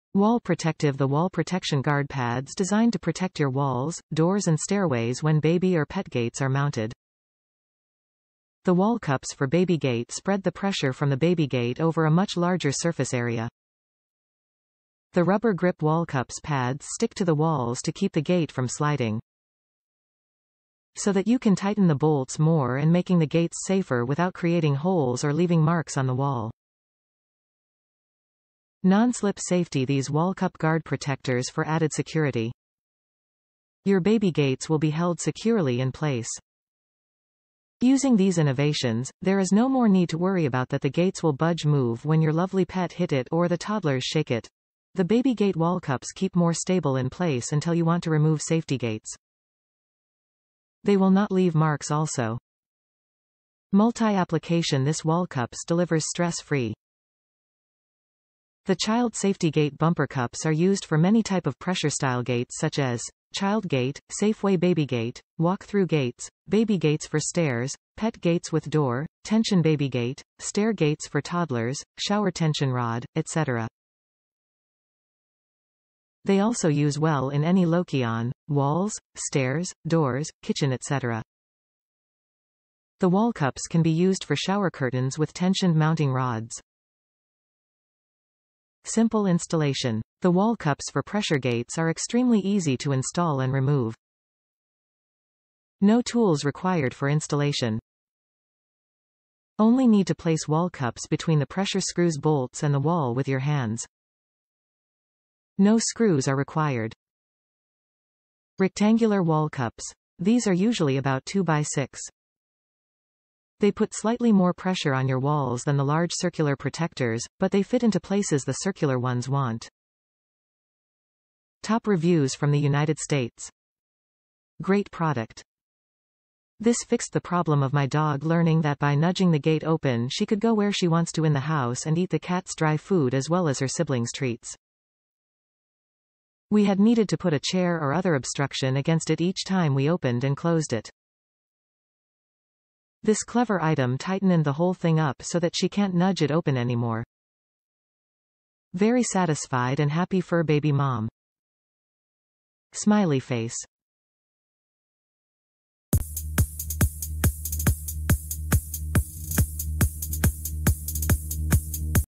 Baby Gates Wall Pads (4 Pack Guard) Safety Indoor Gate Wall Protector - Improved Small Compact Wall
【WALL PROTECTIVE】The wall protection Guard pads designed to protect your walls, doors and stairways when baby or pet gates are mounted. The wall Cups for baby gate spread the pressure from the baby gate over a much larger surface area . The rubber grip wall Cups pads stick to the walls to keep the gate from sliding. So that you can tighten the bolts more and making the gates safer without creating holes or leaving marks on the wall.
【NON-SLIP & SAFETY】These wall Cup guard protectors for added security. Your baby gates will be held securely in place. Using these innovations, There is no more need to worry about that the gates will budge move when your lovely pet hit it or the toddlers shake it.The baby gate wall cups keep more stable in place until you want to remove safety gates. They will not leave marks also.
【MULTI-APPLICATION】This wall cups delivers stress-free. The child safety gate bumper cups are used for many type of pressure style gates such as: child gate, safeway baby gate，walk through gates, baby gates for stairs, pet gates with door, tension baby gate, stair gates for toddlers, shower tension rod, etc.. They also use well in any locaion: walls, stairs, doors, kitchen etc.. The wall cups can be used for shower curtains with tensioned mounting rods.
【SIMPLE INSTALLATION】 The wall cups for pressure gates are extremely easy to install and remove. No tools required for installation. Only need to place wall cups between the pressure screws bolts and the wall with your hands. No screws are required.
【Rectangular Wall Cups】: These are usually about 2" x 6". They put slightly more pressure on your walls than the large circular protectors, but they fit into places the circular ones won't.
Great product
This fixed the problem of my dog learning that by nudging the gate open she could go where she wants to in the house and eat the cat's dry food as well as her siblings treats. We had needed to put a chair or other obstruction against it each time we opened and closed it. This clever item tightenened the whole thing up so that she can't nudge it open anymore. Very satisfied and happy fur baby mom. :)